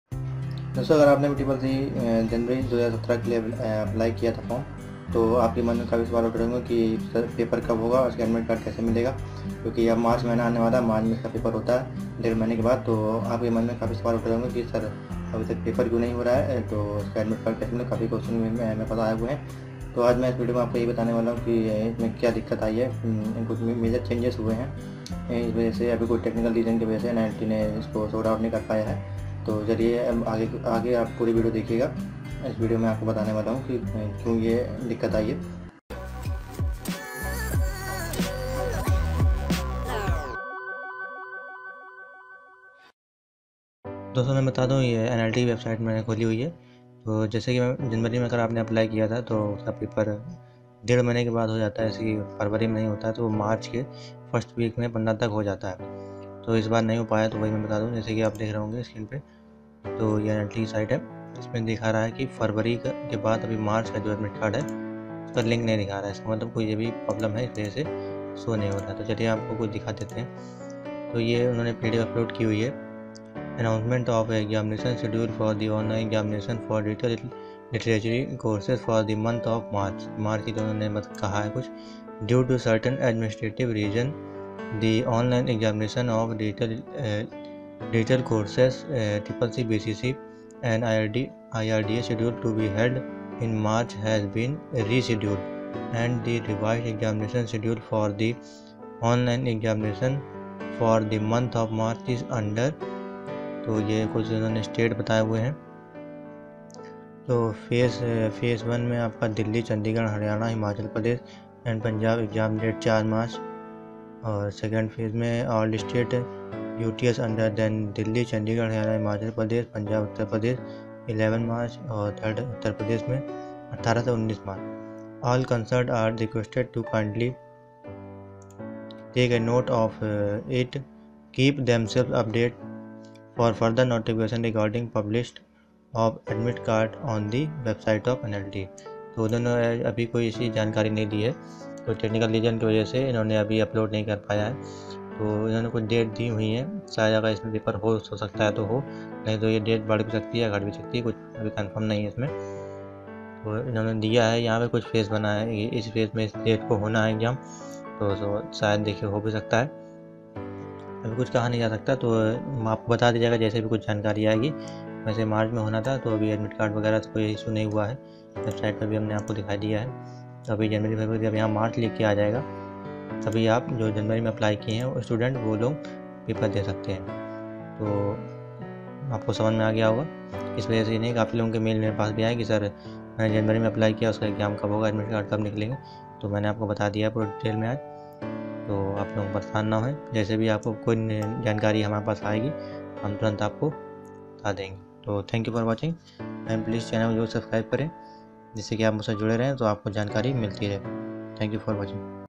CCC Doeacc January 2017 Paper And Admit Card Detail In Hindi
Namaskar dosto is video me maine batay hai ki ccc exam form jinhone january 2017 me apply kiya tha to uska paper kab hoga or admit card kam milega CCC 2017 March exam me jo paper hona hai January apply student ke liye?

__/LINKS\_